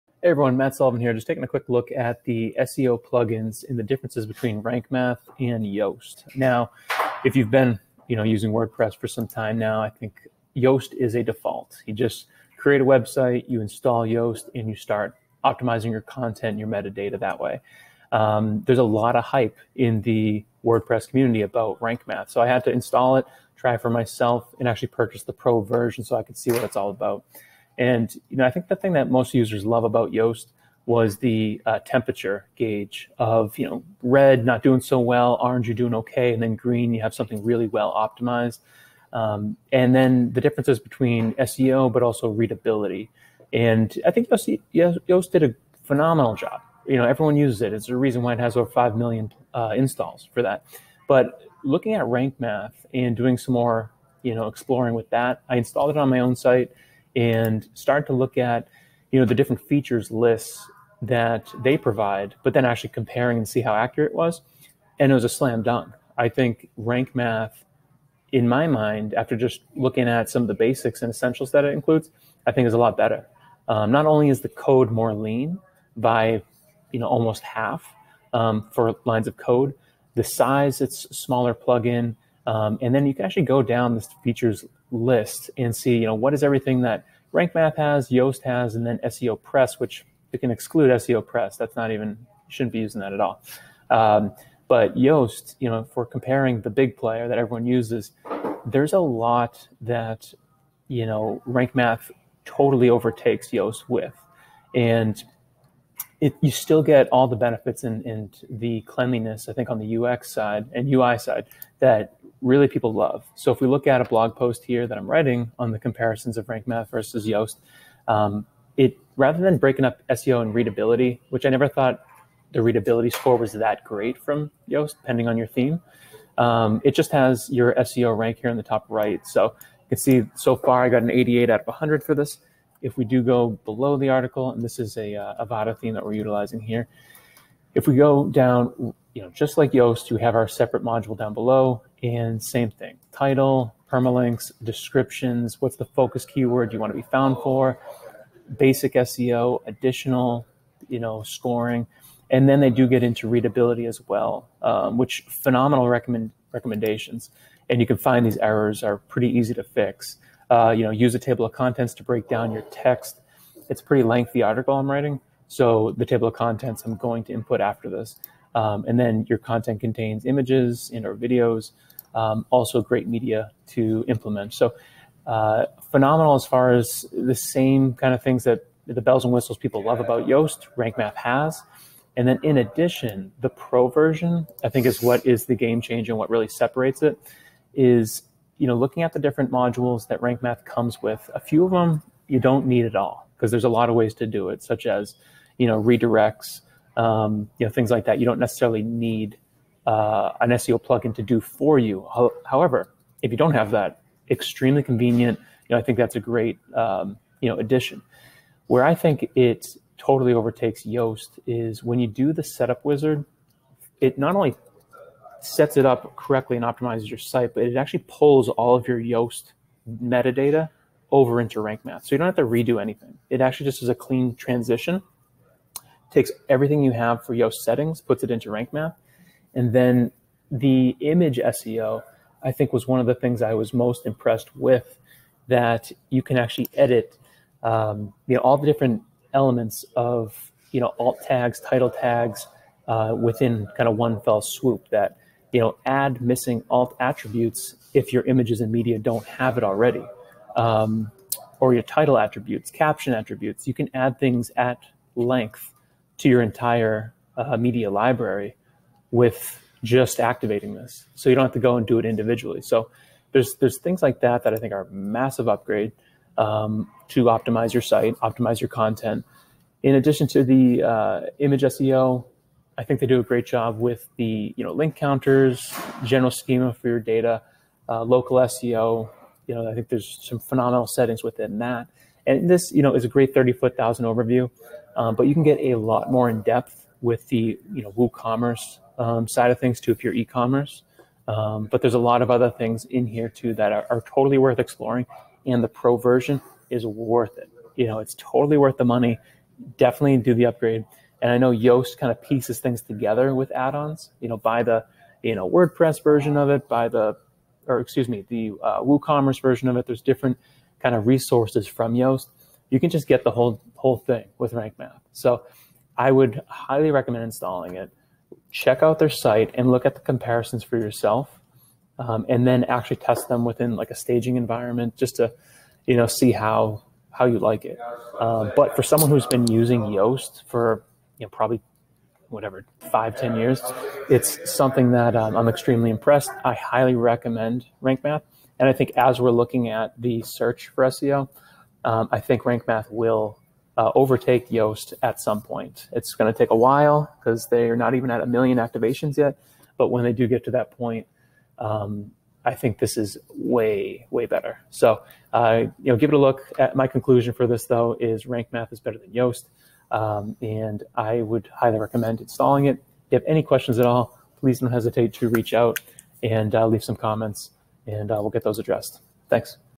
Rank Math vs Yoast SEO: Which Wordpress SEO Plugin is Better?
There are seemingly endless WordPress SEO plugins out there, all promising to boost the visibility of your content. Two at the top of the recommendations list are Yoast SEO and Rank Math. Yoast SEO has remained at the top of the class pretty much since the creation of WordPress, but a new challenger, Rank Math, might just topple its crown.

Both popular plugins have something to offer those looking to boost their SEO. Here’s a review of what both products have to offer those looking to improve their search engine optimization: